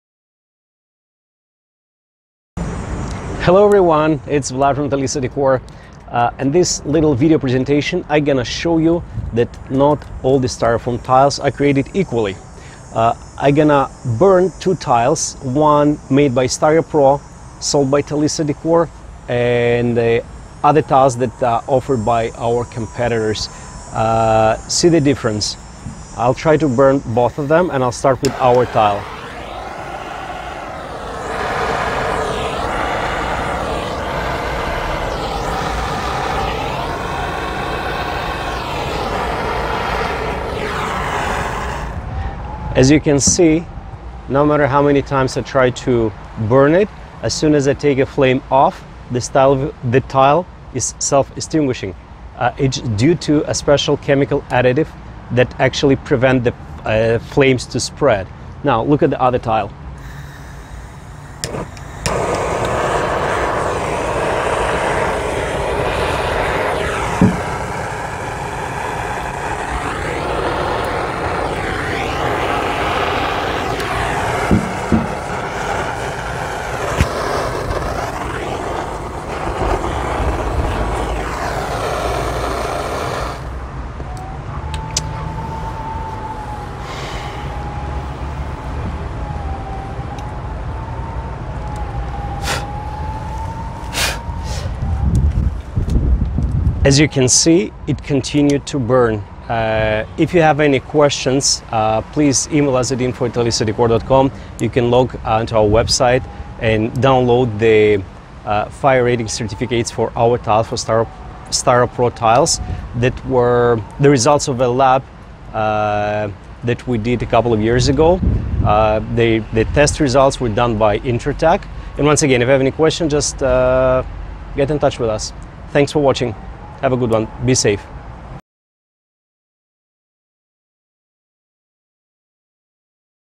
Are Ceiling Tiles Fire Proof? Comparison from Talissa Decor - Ceiling Tiles Fire Rating
Here at Talissa Decor, we take safety very seriously. That's why we strive to provide our customer with as much information about the safety as possible. All of our products are fire rated, which were tested and certified by Intertek Testing Services.

Discover a whole new world of decorative materials with Talissa Decor! Ceiling tiles, wall panels, wall murals, backsplash panels, crown moldings and more!